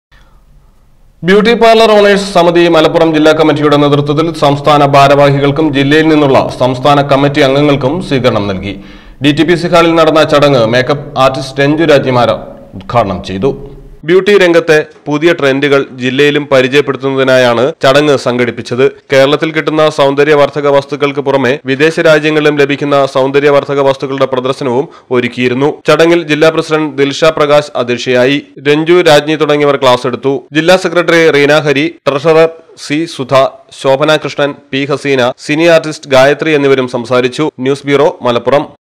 ബ്യൂട്ടി പാര്‍ലര്‍ ഒണേഴ്‌സ് സമിതി സ്വീകരണം
ബ്യൂട്ടി പാര്‍ലര്‍ ഒണേഴ്‌സ് സമിതി മലപ്പുറം ജില്ലാ കമ്മിറ്റിയുടെ നേതൃത്വത്തില്‍  സ്വീകരണം